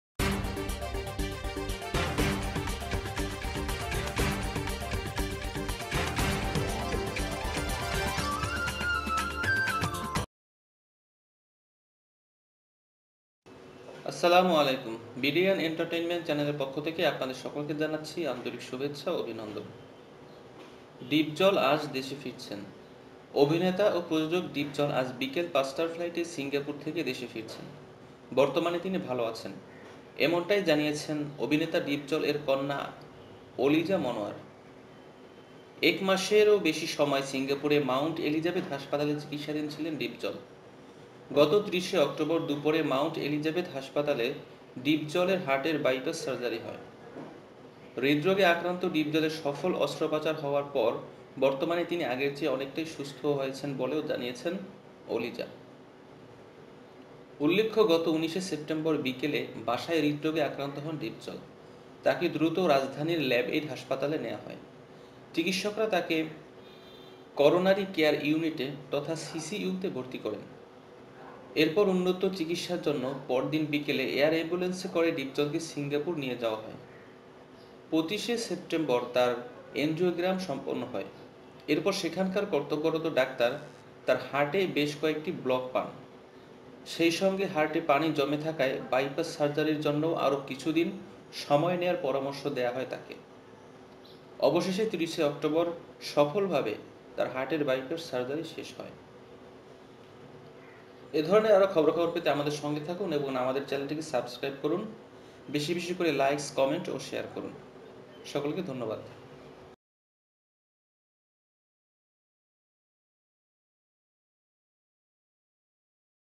ডিপজল আজ দেশে ফিরছেন. Dipjol latest hit news
Actor and producer Dipjal is returning from Singapore on 5 o'clock this afternoon. Now he is good. According to this, Aliza Monwar, the daughter of actor Deepoz Dupjal was undergoing treatment for Mount Elizabeth Hospital in Singapore for more than a month. On October 30, at the Mount Elizabeth Hospital, there was a heart failure bypass surgery. Aliza, who has been diagnosed with heart disease, has been recovering better than ever before, after having a successful surgery.
It is to be noted that Dipjal was suffering from heart disease at his home on September 19 afternoon. He was taken to the capital's Labaid Hospital. The doctors admitted him to the Coronary Care Unit (CCU). After this, Dipjal was taken to Singapore for air treatment in the air ambulance next afternoon for better treatment. On September 25, he completed his NGO. Then the doctor at that place got several blocks in his heart. As well as having heart water, it is suggested to take more time for bypass surgery. Finally, on 30th October, he successfully undergoes heart problems.